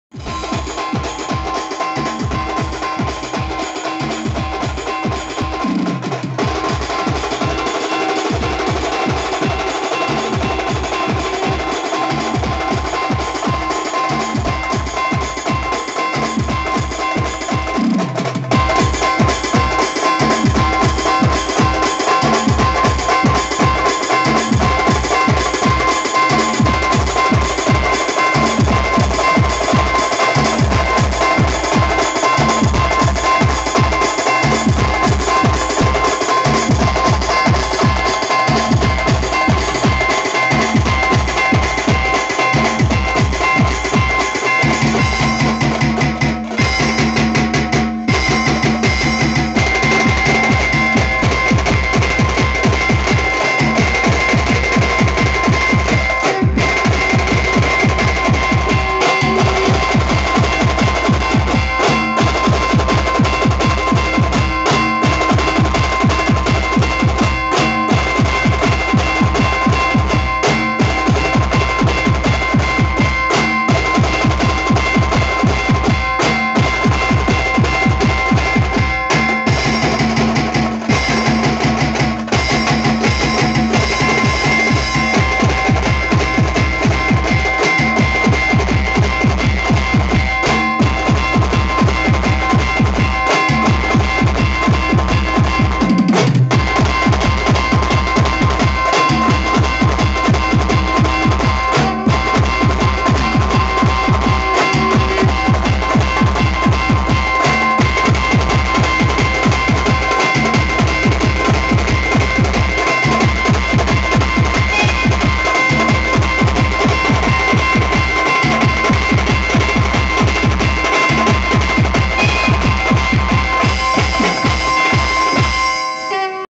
dhol tasha mix pawri maharashtra samrat brass band soegaon aurangabad
full hard bass mix by dhol tasha and mix sambal music by maharashtra samrat brass band soegaon aurangabad full kdkkk brass band .and plizz use to earphones and enjoy the misic. 🙏🙏